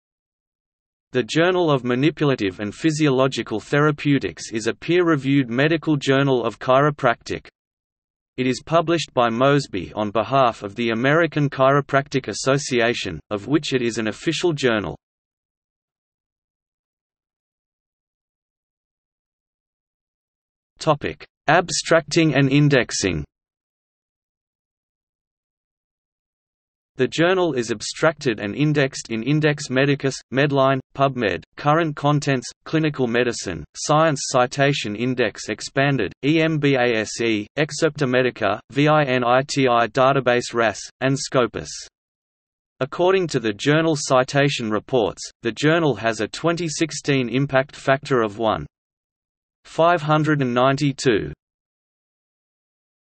Journal of Manipulative and Physiological Therapeutics | Wikipedia audio article
00:00:17 Abstracting and indexing

- Socrates

SUMMARY
The Journal of Manipulative and Physiological Therapeutics is a peer-reviewed medical journal of chiropractic. It is published by Mosby on behalf of the American Chiropractic Association, of which it is an official journal.